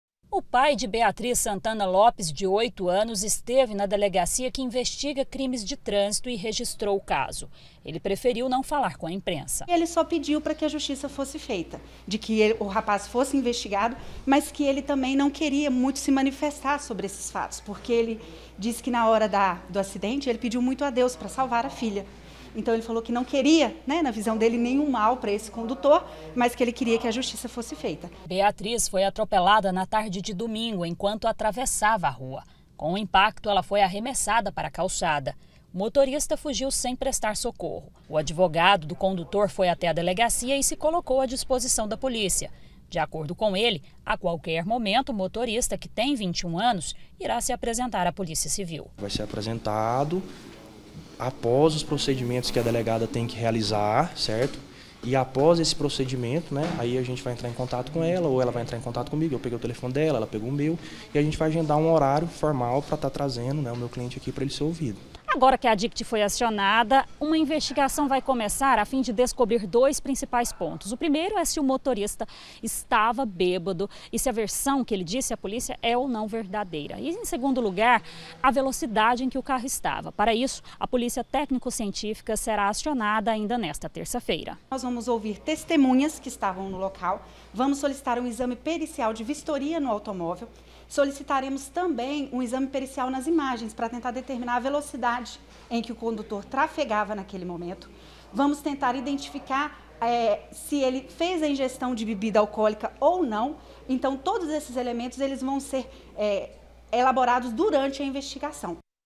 CRIANÇA ATROPELADA: MOTORISTA DEVE SER OUVIDO NOS PRÓXIMOS DIAS
A POLÍCIA CIVIL ABRIU INQUÉRITO PARA INVESTIAR O ATROPELAMENTO DE UMA MENINA DE OITO ANOS, ENQUANTO ATRAVESSAVA A RUA. A FAMÍLIA DA CRIANÇA JÁ FOI OUVIDA.